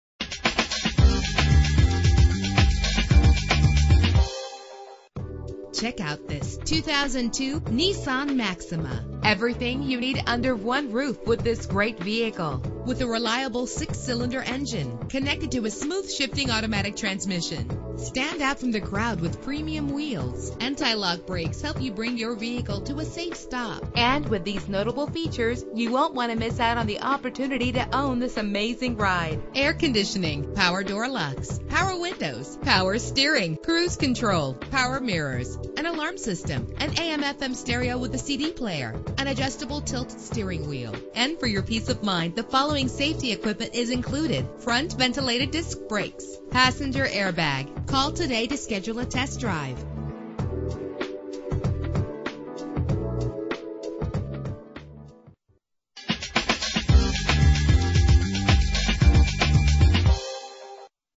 2002 Nissan Maxima - Crystal Lake IL
Year: 2002
Make: Nissan
Model: Maxima
Trim: SE
Engine: 3.5 liter 6 cylinder 24 valve
Transmission: 4-Speed Automatic
Color: Sunlit Sand
Mileage: 85756
Address:
5656 Northwest Hwy